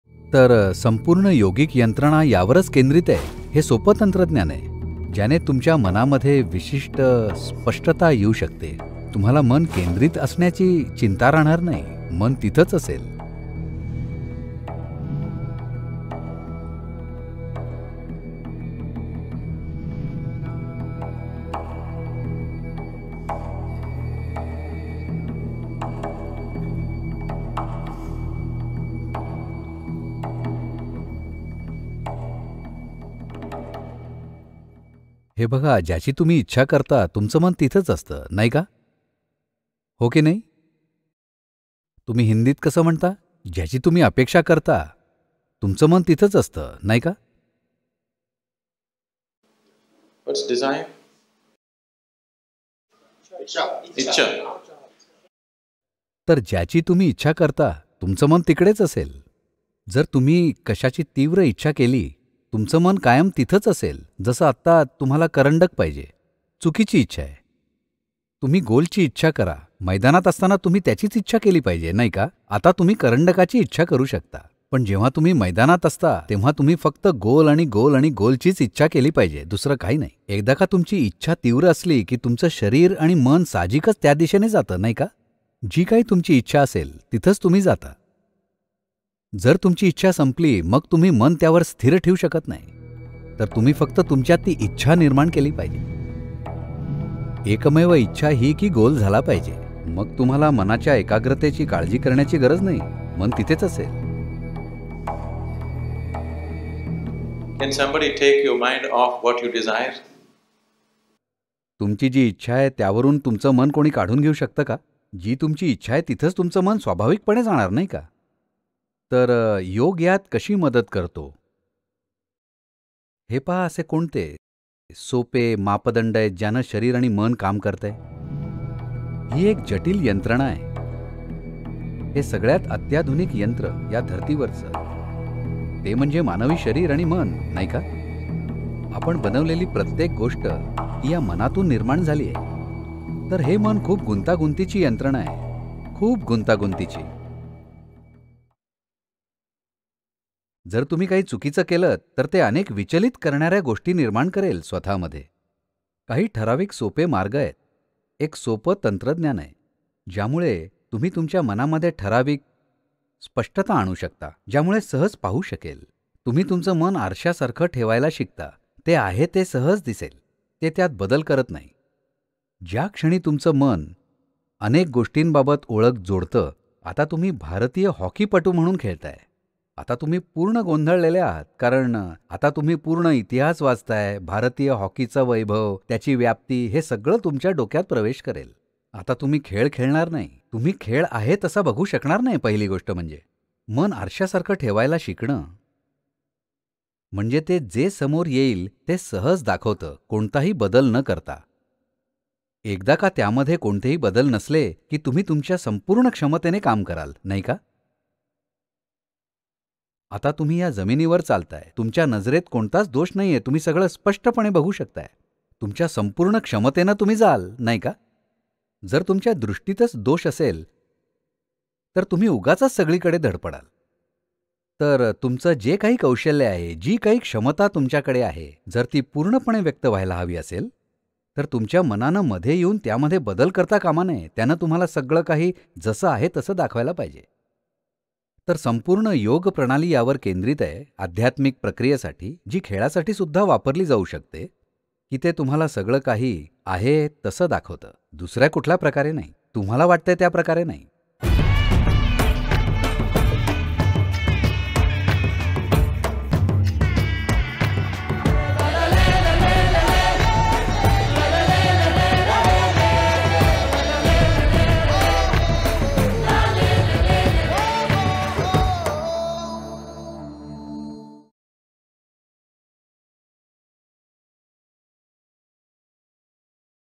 इच्छा आणि स्वप्न खरी करण्याचा मार्ग | The Secret To Manifest What You Want - Sadhguru Marathi
आयुष्यात हवं ते साध्य करण्याचा मार्ग - सद्गुरू

English Video - The Secret To Manifest What You Want | Sadhguru